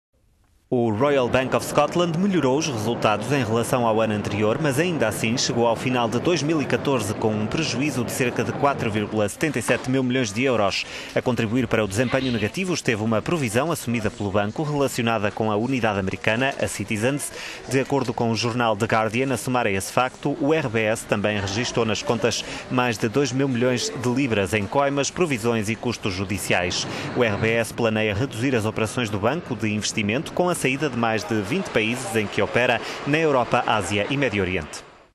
RBS continua a apresentar perdas, mas de forma mais moderada - economy
O Royal Bank of Scotland (RBS) melhorou os resultados em relação ao ano anterior mas, ainda assim, chegou ao final de 2014 com um prejuízo de 3,5 mil milhões de libras, o equivalente a 4,77 mil milhões de euros.

A contribuir para o desempenho negativo esteve uma provisão assumida pelo banco relacionada com a unidade americana, a Citizens.

De acordo com o jornal "The Guardian", a somar a esse facto, o RBS também registou nas contas mais de dois mil milhões de libras em coimas, provisões e custo…